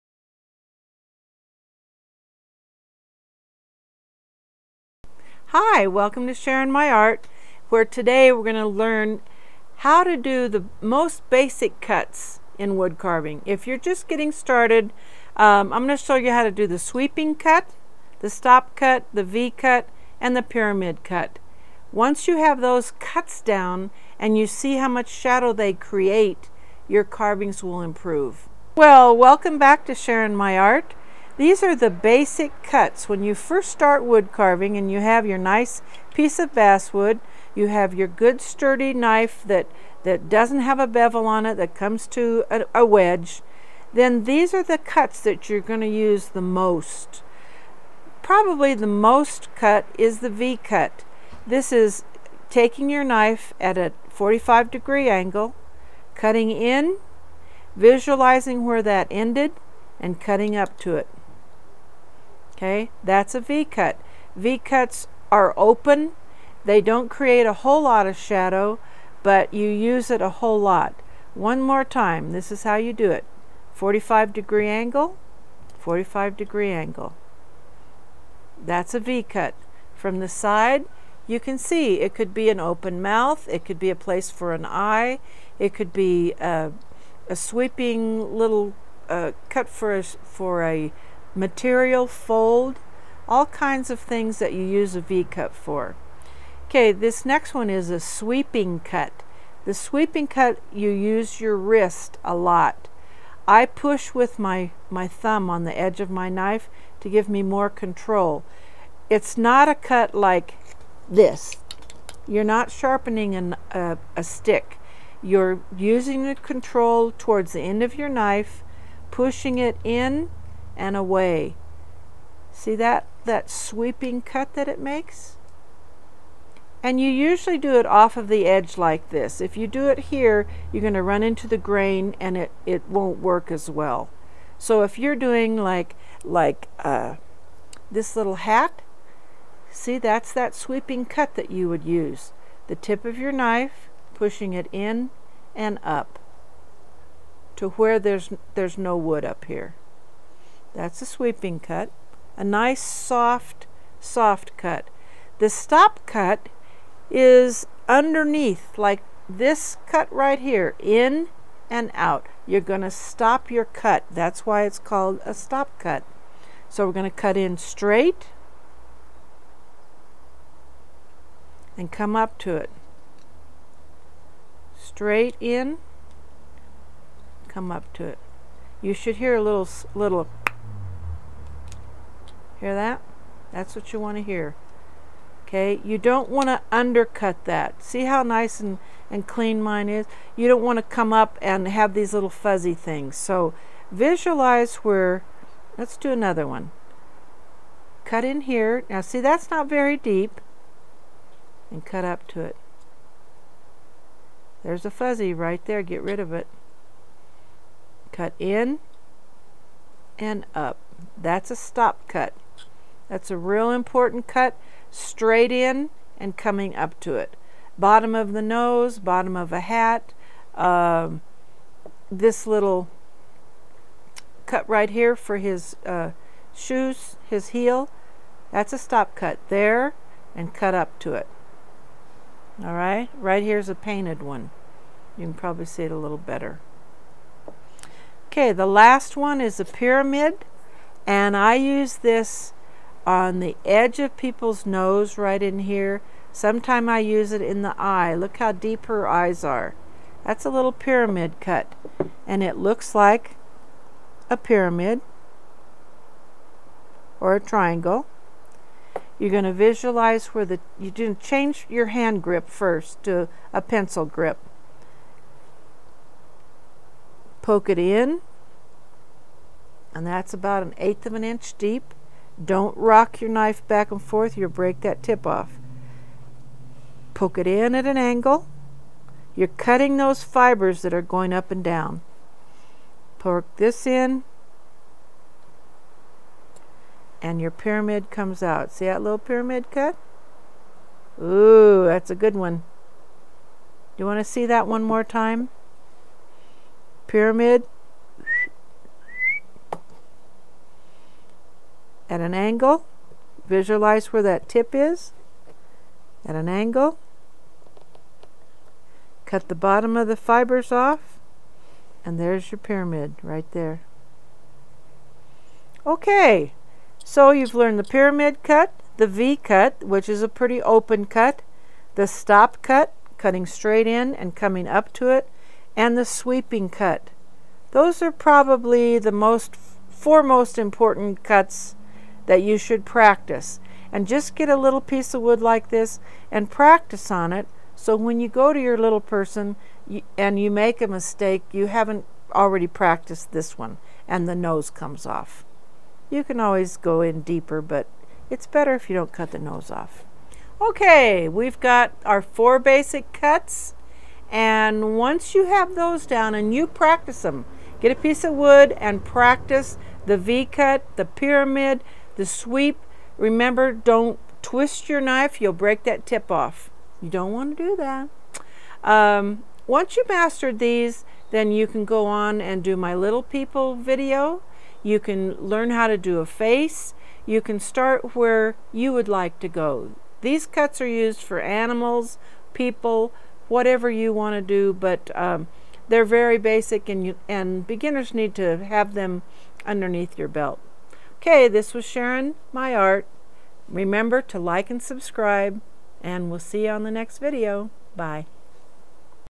Wood Carving 4 Basic Cuts to master
In this video you will learn the 4 basic cuts that you will need to learn and master in wood carving. You will use these same cuts for everything you carve. Make sure you practice before starting your own carvings.